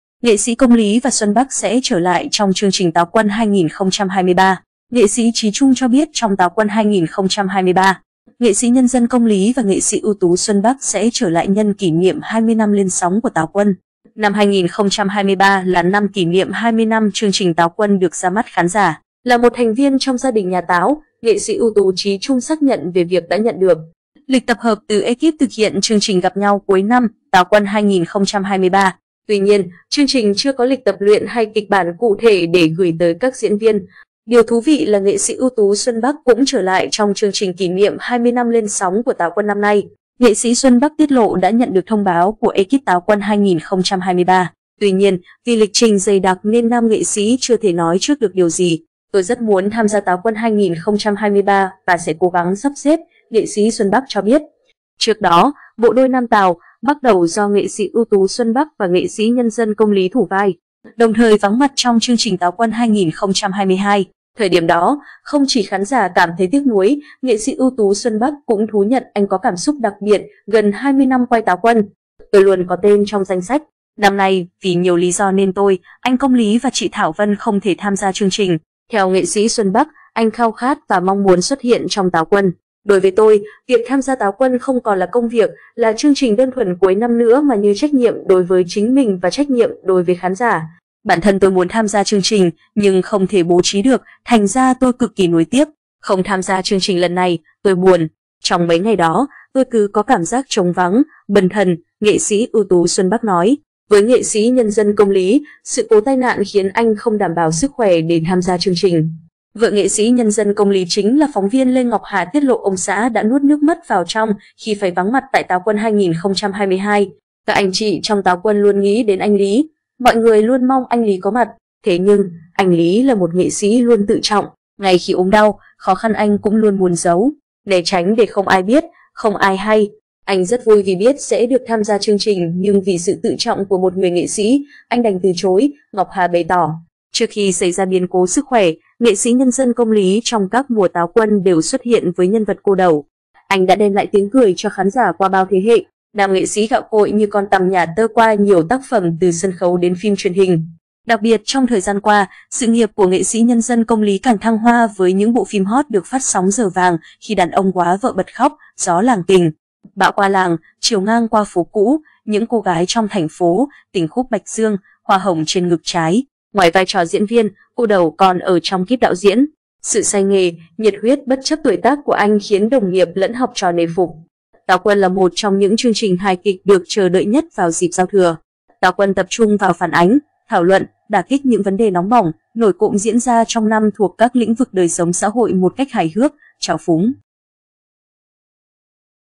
Nghệ sĩ Công Lý và Xuân Bắc sẽ trở lại trong chương trình Táo quân 2023
Nghệ sĩ Chí Trung cho biết trong Táo quân 2023, NSND Công Lý và NSƯT Xuân Bắc sẽ trở lại nhân kỷ niệm 20 năm lên sóng của Táo quân.Trước khi xảy ra biến cố sức khỏe, NSND Công Lý trong các mùa Táo quân đều xuất hiện với nhân vật "cô Đẩu". Anh đã đem lại tiếng cười cho khán giả qua bao thế hệ. Nam nghệ sĩ gạo cội như "con tằm nhả tơ" qua nhiều tác phẩm từ sân khấu đến phim truyền hình. Đặc biệt trong thời gian qua, sự nghiệp của NSND Công Lý càng thăng hoa với những bộ phim hot được phát sóng giờ vàng: Khi đàn ông góa vợ bật khóc, Gió làng Kình, Bão qua làng, Chiều ngang qua phố cũ, Những cô gái trong thành phố, Tình khúc bạch dương, Hoa hồng trên ngực trái... Ngoài vai trò diễn viên, "cô Đẩu" còn ở trong kíp đạo diễn. Sự say nghề, nhiệt huyết bất chấp tuổi tác của anh khiến đồng nghiệp lẫn học trò nể phục.
Khán giả vui mừng khi hay tin nghệ sĩ Công Lý sẽ trở lại với Táo quân 2023
Táo quân là một trong những chương trình hài kịch được chờ đợi nhất vào dịp giao thừa. Táo Quân tập trung vào phản ánh, thảo luận, đả kích những vấn đề nóng bỏng, nổi cộm diễn ra trong năm thuộc các lĩnh vực đời sống xã hội một cách hài hước, trào phúng.
Điều thú vị là NSƯT Xuân Bắc cũng trở lại trong chương trình kỷ niệm 20 năm lên sóng của Táo quân năm nay. Nghệ sĩ Xuân Bắc tiết lộ đã nhận được thông báo của ê-kíp Táo quân 2023. Tuy nhiên, vì lịch trình dày đặc nên nam nghệ sĩ chưa thể nói trước được điều gì. “Tôi rất muốn tham gia Táo quân 2023 và sẽ cố gắng sắp xếp”, nghệ sĩ Xuân Bắc cho biết.
Trước đó, bộ đôi Nam Tào – Bắc Đẩu do NSƯT Xuân Bắc và NSND Công Lý thủ vai đồng thời vắng mặt trong chương trình Táo quân 2022. Thời điểm đó, không chỉ khán giả cảm thấy tiếc nuối, NSƯT Xuân Bắc cũng thú nhận anh có cảm xúc đặc biệt: “Gần 20 năm quay Táo quân, tôi luôn có tên trong danh sách. Năm nay vì nhiều lý do nên tôi, anh Công Lý và chị Thảo Vân không thể tham gia chương trình”.
Năm 2023 là năm kỷ niệm 20 năm chương trình Táo quân được ra mắt khán giả. Là một thành viên trong "gia đình nhà Táo" - NSƯT Chí Trung xác nhận về việc đã nhận được lịch tập hợp từ ê-kíp thực hiện chương trình Gặp nhau cuối năm – Táo quân 2023. Tuy nhiên, chương trình chưa có lịch tập luyện hay kịch bản cụ thể để gửi tới các diễn viên.
Theo nghệ sĩ Xuân Bắc, anh khao khát và mong muốn xuất hiện trong Táo quân. “Đối với tôi, việc tham gia Táo quân không còn là công việc, là chương trình đơn thuần cuối năm nữa mà như trách nhiệm đối với chính mình và trách nhiệm đối với khán giả. Bản thân tôi muốn tham gia chương trình, nhưng không thể bố trí được, thành ra tôi cực kỳ nuối tiếc. Không tham gia chương trình lần này, tôi buồn. Trong mấy ngày đó, tôi cứ có cảm giác trống vắng, bần thần”, NSƯT Xuân Bắc nói.
Với NSND Công Lý, sự cố tai nạn khiến anh không đảm bảo sức khỏe để tham gia chương trình. Vợ NSND Công Lý chính là phóng viên Lê Ngọc Hà tiết lộ ông xã đã “nuốt nước mắt vào trong” khi phải vắng mặt tại Táo quân 2022. “Các anh chị trong Táo Quân luôn nghĩ đến anh Lý. Mọi người luôn mong anh Lý có mặt. Thế nhưng, anh Lý là một nghệ sĩ luôn tự trọng. Ngay khi ốm đau, khó khăn anh cũng luôn muốn giấu. Né tránh để không ai biết, không ai hay. Anh rất vui vì biết sẽ được tham gia chương trình nhưng vì sự tự trọng của một người nghệ sĩ, anh đành từ chối”, Ngọc Hà bày tỏ.